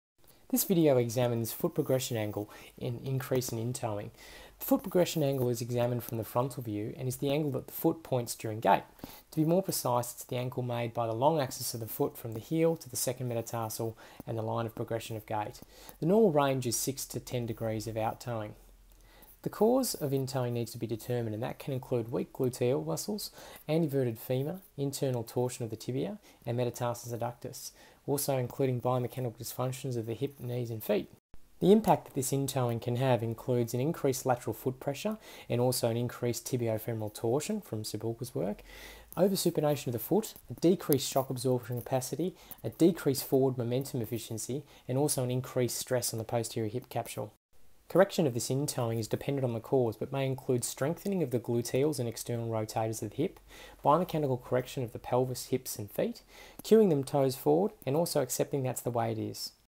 Enhance Running: foot progression angle in toeing
This video examines Foot progression angle: increased in toeing

- The foot progression angle is examined from the frontal view and is the angle that the foot points during gait. To be more precise, it’s the angle made by the long axis of the foot from the heel to the 2nd metatarsal, & the line of progression of gait
- Normal range is 6-10 degrees of out toeing.

The cause needs to be determined, which can include:
- Weak Glutes & external hip rotators
- Anteverted femur, internal torsion tibia, metatarsus adductus
- Biomechanical dysfunction of Hips, knees, Feet

Considerations:
- Increased lateral foot pressure, & also associated with increased tibiofemoral torsion (Cibulka, M. T., et al. 2016)
- Oversupination of the foot
- Decreased shock absorption capacity
- Decreased forward momentum efficiency
- Increased stress on posterior hip capsule

Correction
- Dependant on the CAUSE but may include:
- Glute/external rotator strengthening
- Biomechanical correction of Pelvis/Hips/Feet
- Cue toes forward- caution with this
- Accept that’s the way it is!!

References
Cibulka, M. T., et al. (2016). "PREDICTING FOOT PROGRESSION ANGLE DURING GAIT USING TWO CLINICAL MEASURES IN HEALTHY ADULTS, A PRELIMINARY STUDY." Int J Sports Phys Ther 11(3): 400-408.

Follow Enhance Running on
1. Instagram @Enhance_Running
3. YouTube